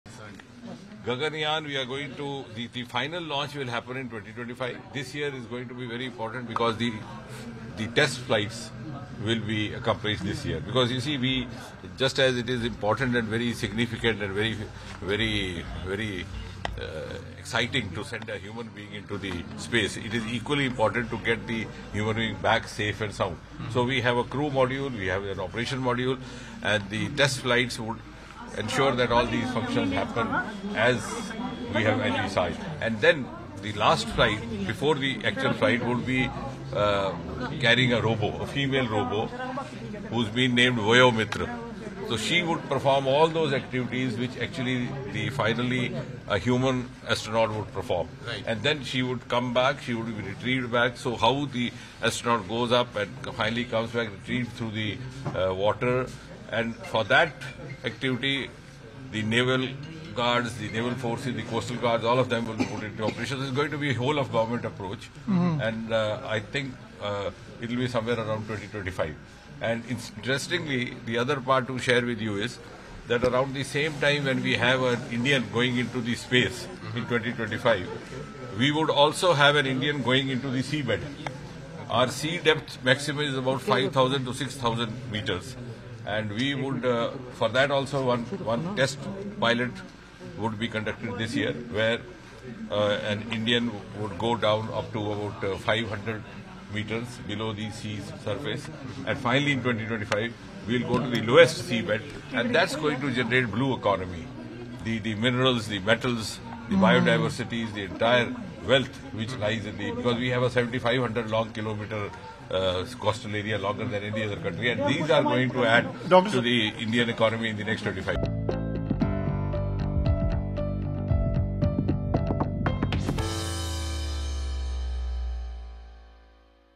ISRO to carry out final launch of ‘Gaganyaan’ in 2025: Jitendra Singh
Speaking about the ISRO's Gaganyaan Project, Union Minister of State (Ind. Charge) Science & Technology Jitendra Singh informed on January 07 that ISRO is going to the final launch in the year 2025.While speaking to the media, Union Minister of state Jitendra Singh said, "We are going to the final launch in 2025. This year is very important because the test flights will be done this year. It is important and exciting to send a human being into space, it is equally important to get the human being back safe and sound. We have a crew module and an operation module and the test flights would ensure that all these functions happen as we have envisaged. The last flight before the action flight would be carrying a female robo named 'Voyo Mitra'. She would perform all those activities which a human astronaut would perform... It's going to be a whole of government approach... Around the same time when we have an Indian going into space in 2025, we will also have an Indian going into the sea bed... In 2025, we'll go to the lowest seabed, and that's going to generate a blue economy..."